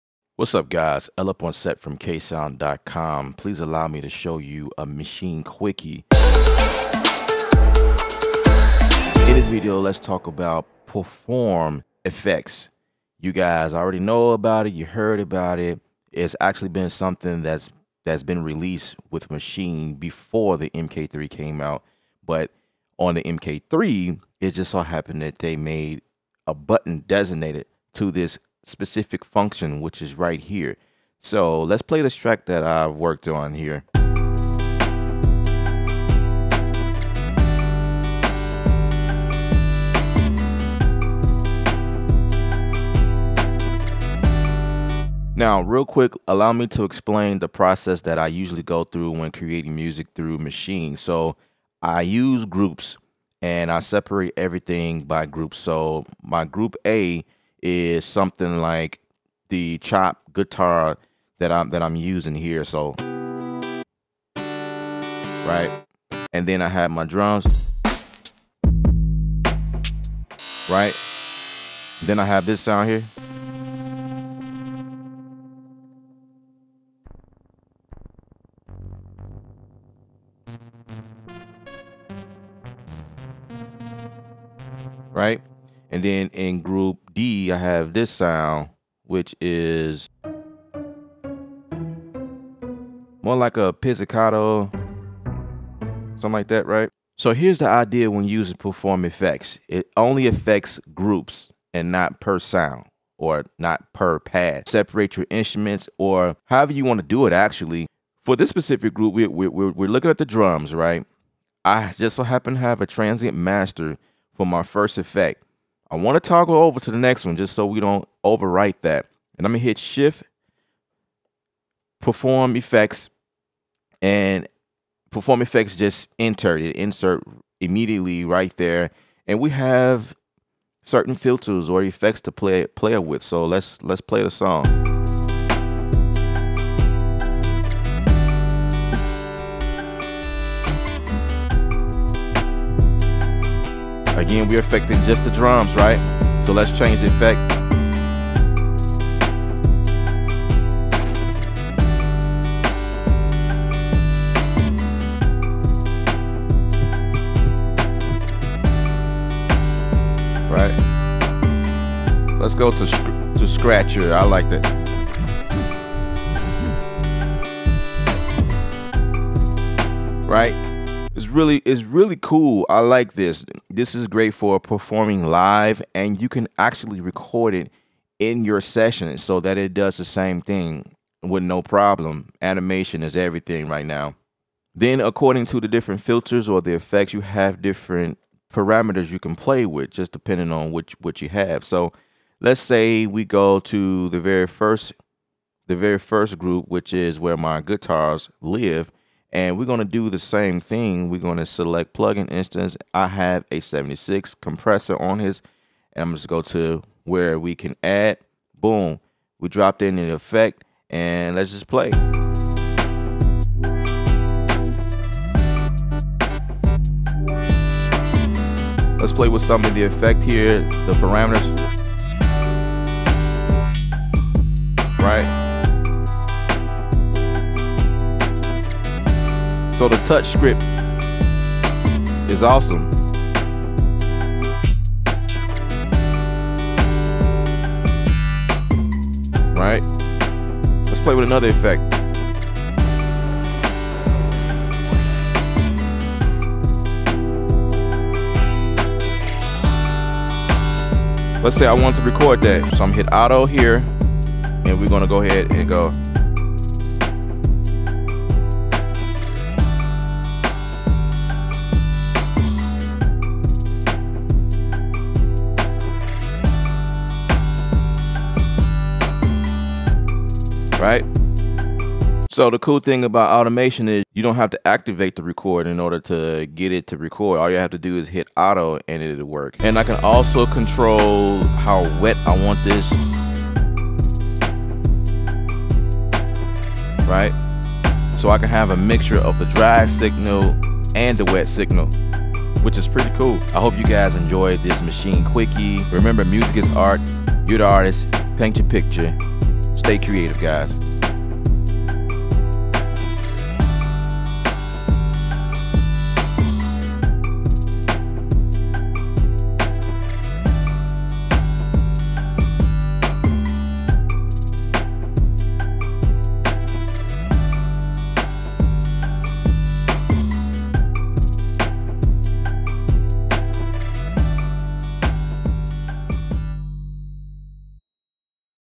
Maschine MK3 Quickie: Perform FX
Create really kool effects with Perform FX in Native Instruments Maschine MK3.